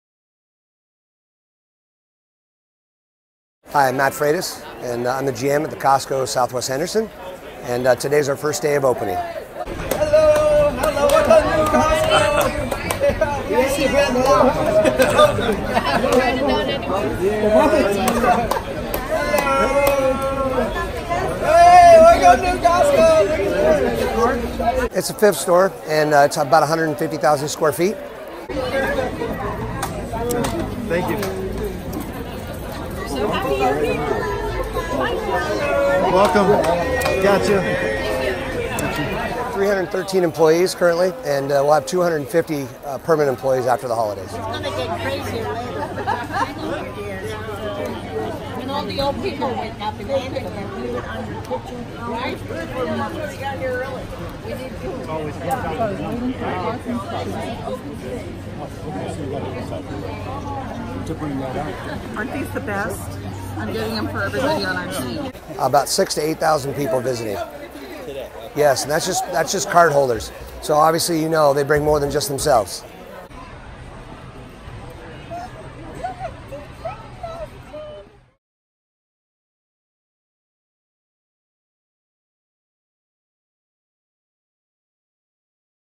Costco opens its doors in southwest Henderson
Costco has opened its fifth Las Vegas-area location near the intersection of St. Rose Parkway and Amigo Street. (K.M. Cannon/Las Vegas Review-Journal)